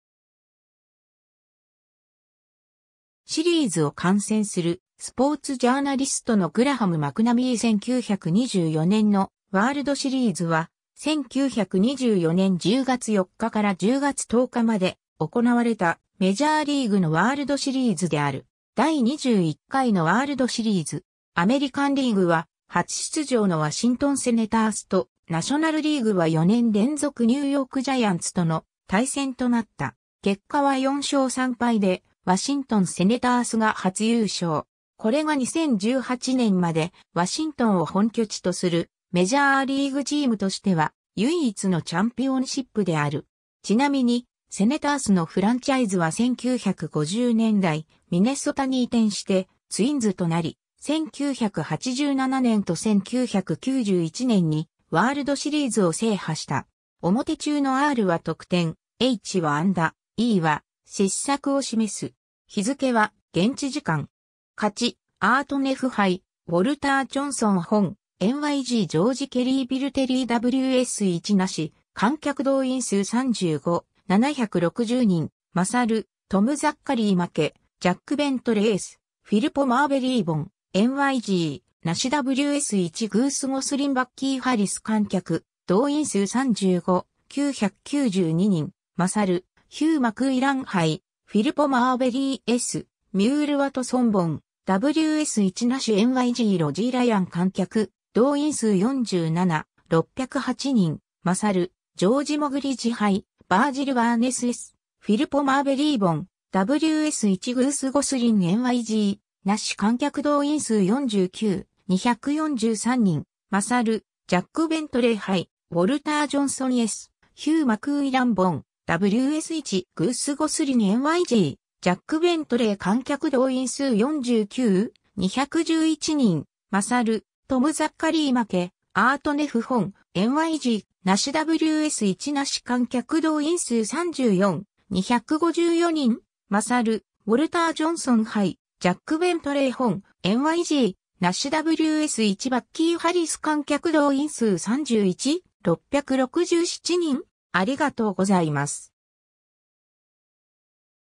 1924年のワールドシリーズ
ミネソタ・ツインズ
サンフランシスコ・ジャイアンツ
シリーズを観戦するスポーツジャーナリストのグラハム・マクナミー 1924年のワールドシリーズは、1924年10月4日から10月10日まで行われたメジャーリーグのワールドシリーズである。
 第21回のワールドシリーズ。
アメリカンリーグは初出場のワシントン・セネタースとナショナルリーグは4年連続ニューヨーク・ジャイアンツとの対戦となった。
結果は4勝3敗でワシントン・セネタースが初優勝。
これが2018年まで、ワシントンを本拠地とするメジャーリーグチームとしては唯一のチャンピオンシップである。
ちなみにセネタースのフランチャイズは1950年代ミネソタに移転してツインズとなり、1987年と1991年にワールドシリーズを制覇した。
 表中のRは得点、Hは安打、Eは失策を示す。
日付は現地時間。